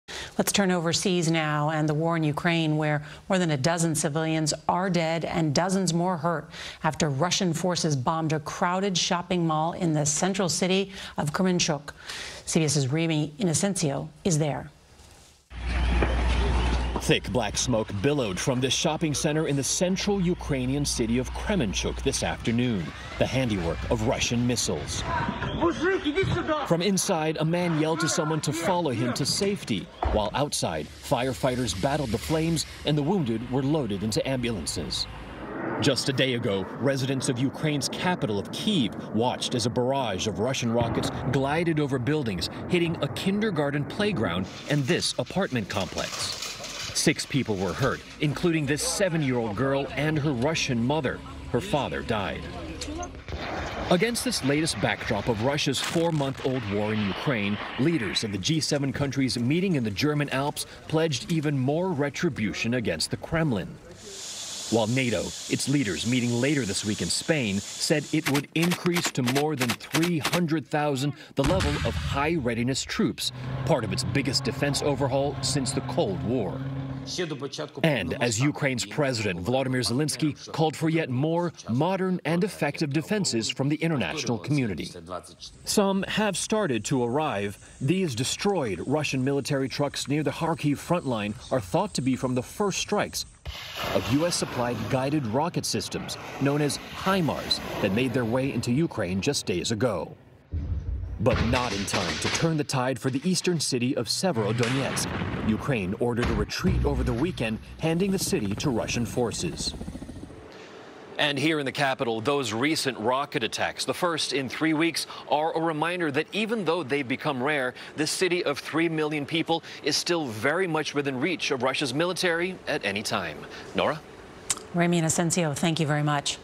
Russian missile strike hits shopping center in Ukraine
Russia launched its first rocket attacks into Ukraine in three weeks, hitting a residential building in Kyiv and a shopping center in Kremenchuck. Meanwhile, G-7 leaders met in Germany and pledged to enact more penalties against Russia. Ramy Inocencio is in Kyiv with the latest.